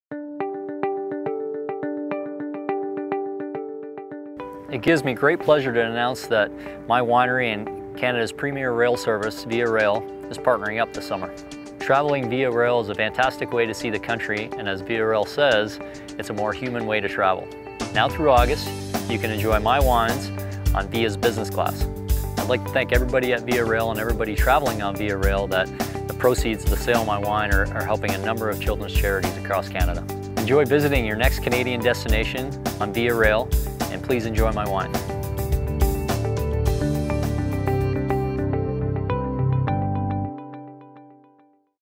Mike Weir Wine - VIA Rail
Now through August you can enjoy my wines while riding VIA Rail business class. Cheers!